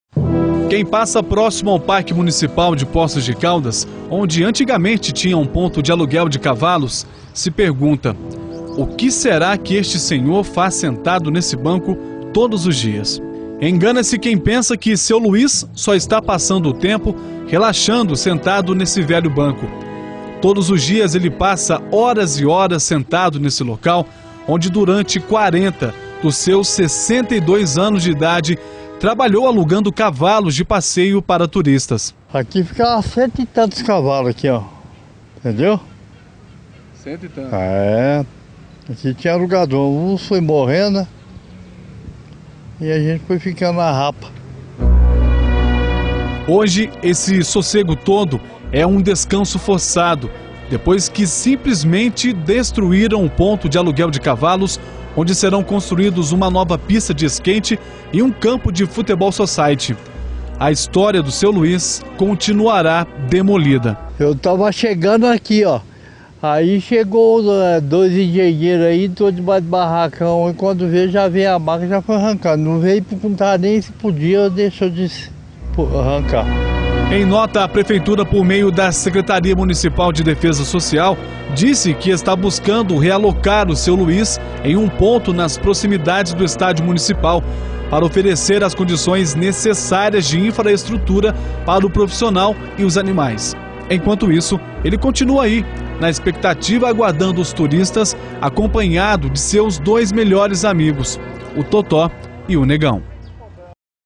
CHARRETISTA SEM TRABALHO
Quem passa próximo ao Parque Municipal de Poços de Caldas, onde antigamente tinha um ponto de aluguel de cavalos, se pergunta: o que será que este senhor faz sentado nesse banco todos os dias?